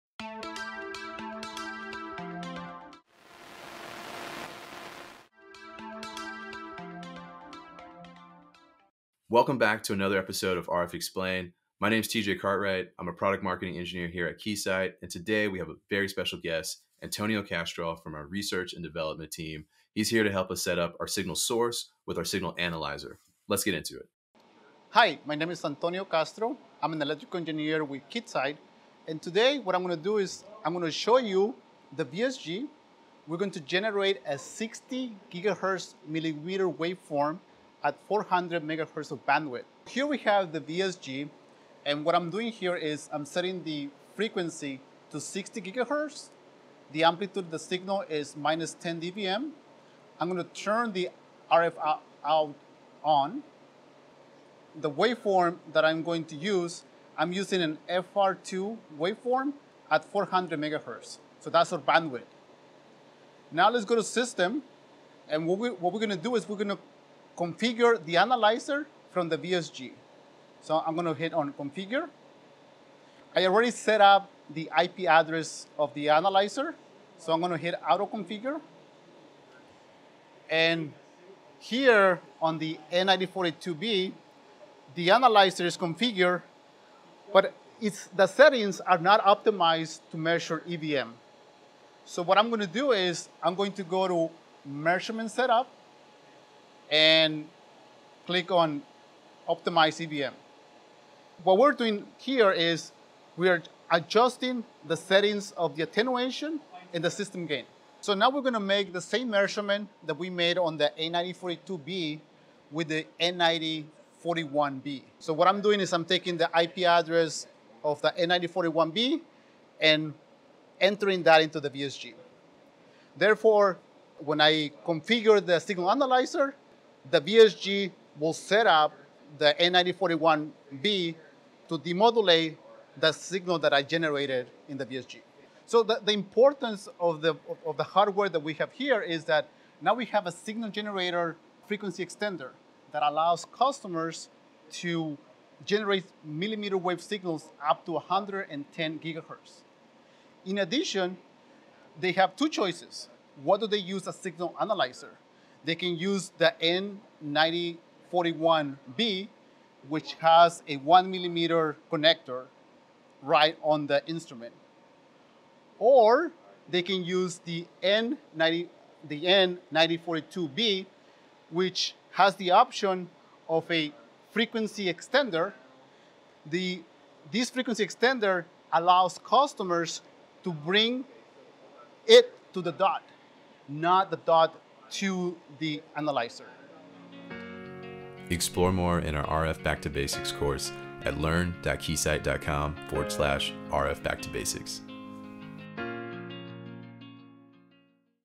RF Explained Episode 5: VXG and UXA mmWave Setup
Welcome to another episode of RF Explain, where we learn about the latest test and measurement instruments for RF engineering. Today we get hands-on setting up our RF instruments to generate and analyze a 60 GHz mmWave. Our special guest Antonio Castro shows us how to set up a signal source with a signal analyzer using the VXG and UXA.

Chapters
00:00 Introduction
00:40 M9484C signal generator setup
01:20 N9042B signal analyzer setup
01:53 N9042B signal analyzer setup
02:22 V3080A frequency extender